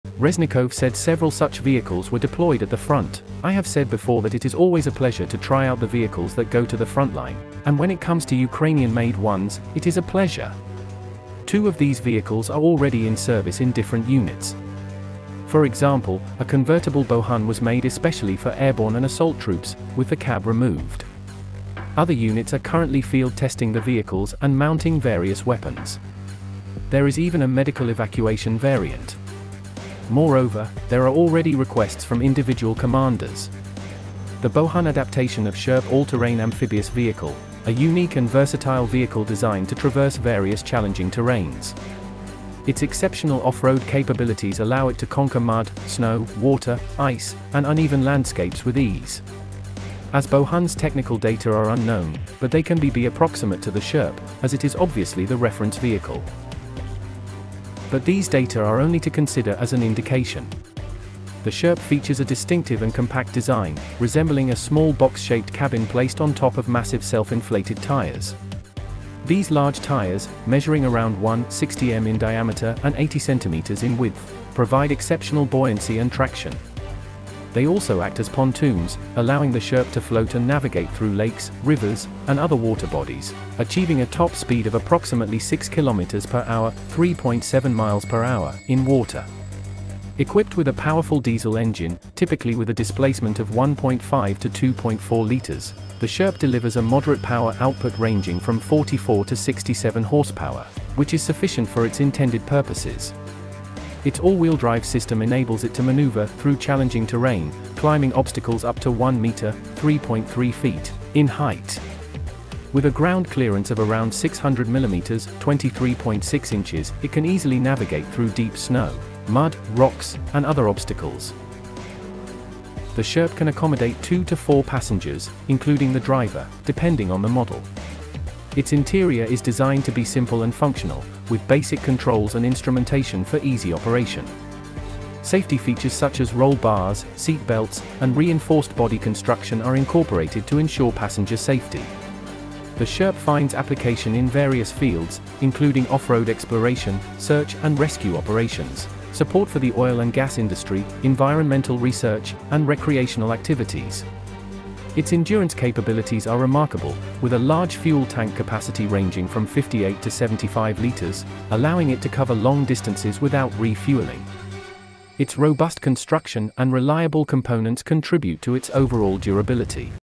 Defence Minister Showed Ukrainian-made Bohun All-terrain Vehicle
Oleksii Reznikov, Ukraine's Defence Minister, has test-driven a Ukrainian-made Bohun all-terrain vehicle